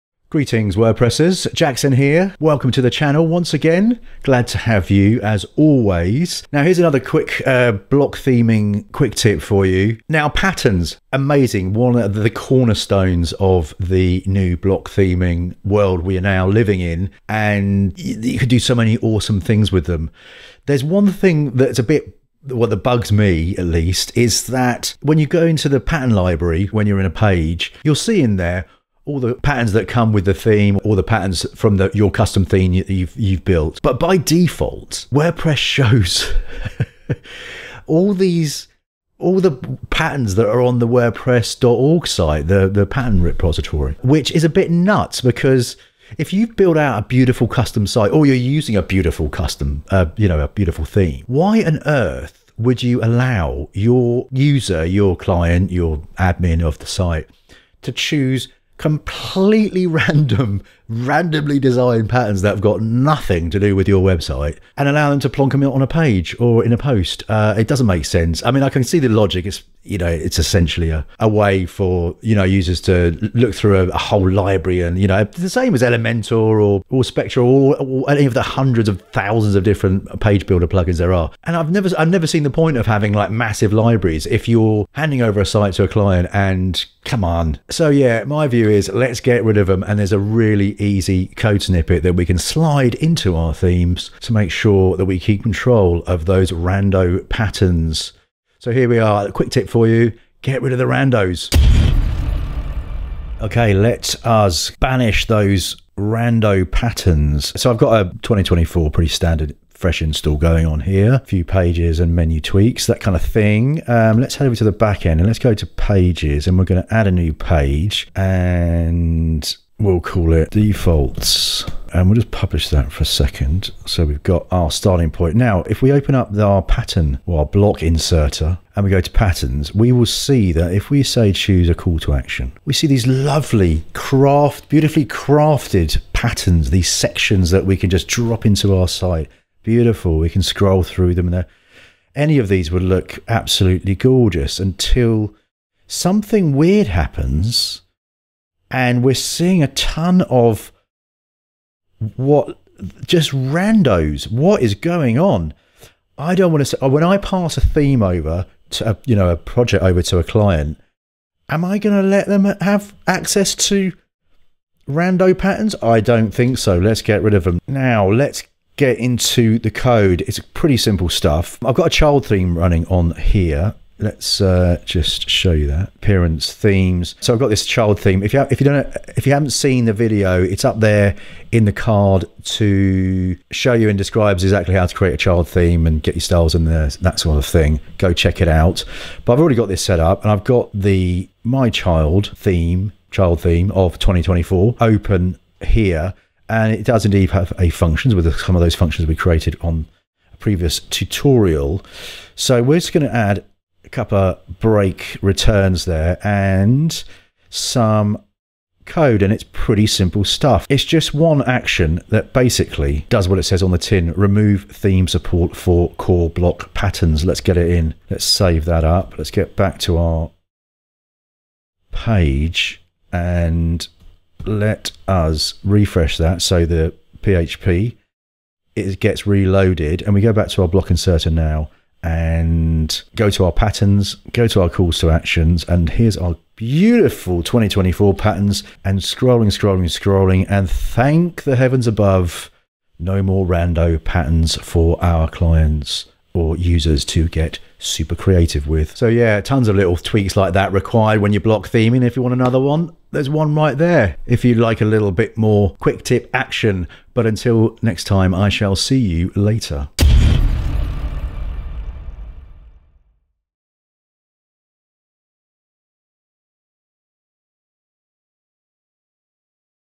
How to disable core patterns in WordPress - No random patterns please!
There’s not much I don’t like about WordPress Block Themes - I use the Full Site Editor and Gutenberg everyday to build websites for my clients.

And Block Patterns, one of the cornerstones of the new block theme world we now live in are amazing, you can do so many awesome things with them.

But… there's one thing that bugs me!

When you go into the pattern library on a page, you'll see all the patterns that come with the theme or the patterns from your custom theme. But by default, WordPress shows all the patterns from the wordpress.org site, from the pattern repository.

This is a bit nuts because if you've built out a beautiful custom site or you're using a beautiful theme, why would you allow your users to choose completely random patterns that have nothing to do with your website?

It doesn't make sense. I can see the logic, essentially a way for users to look through a whole library, similar to Elementor or Spectra or any of the other page builder plugins. But, I've never seen the point of having massive libraries of website parts/sections etc that have got zero to do with your design - crazy!

So yeah, my view is let's get rid of them!

In this Block Theme tutorial  we’ll use a really easy code snippet that we can slide into our themes to make sure that we keep control of those random patterns.

00:00 Patterns are amazing
00:20 The problem of random patterns!
01:41 How to disable core patterns in WordPress
04:09 More Block Theme goodies

Ciao!